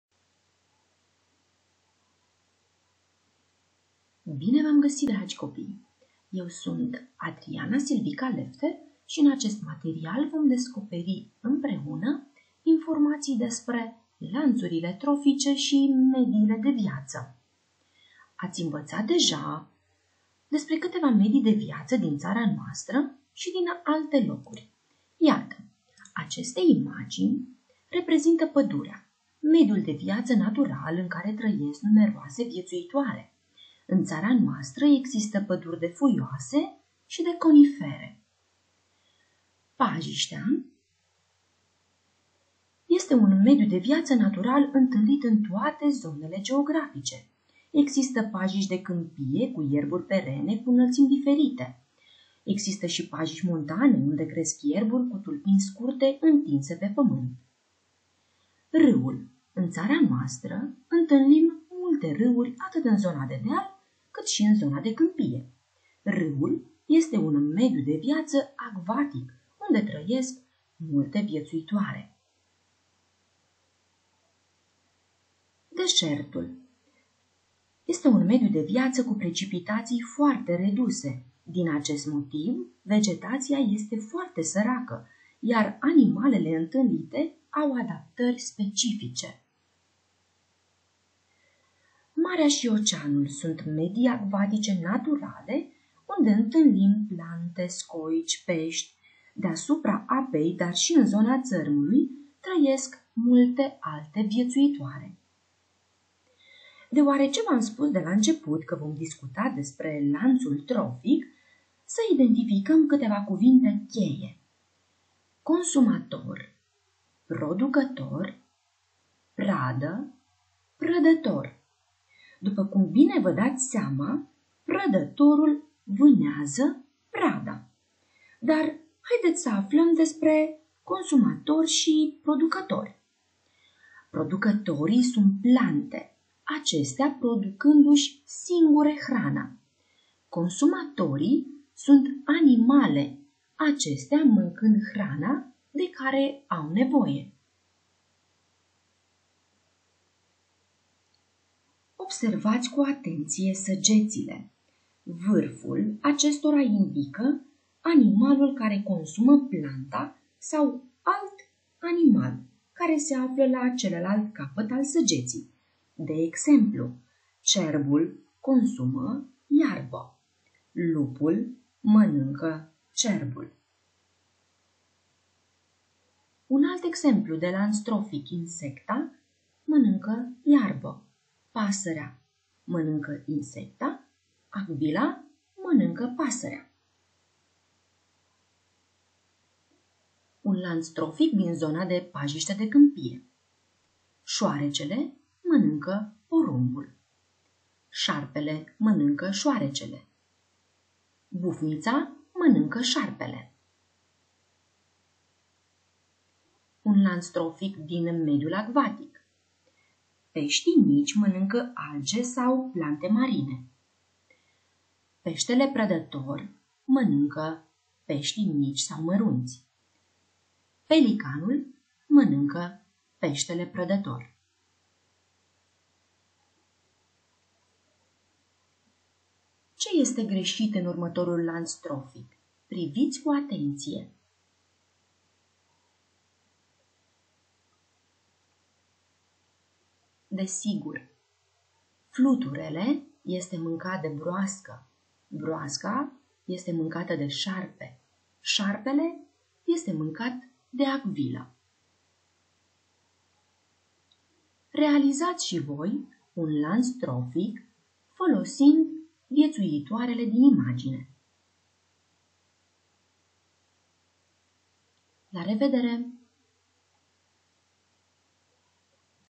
Lanțurile trofice și mediile de viață
Resursa educațională propusă este un material video despre lanțuri trofice și medii de viață. Elevii află în acest video cuvinte cheie, identifică și reprezintă grafic lanțuri trofice pentru diferite medii de viață.

Autor: Adriana Silvica Lefter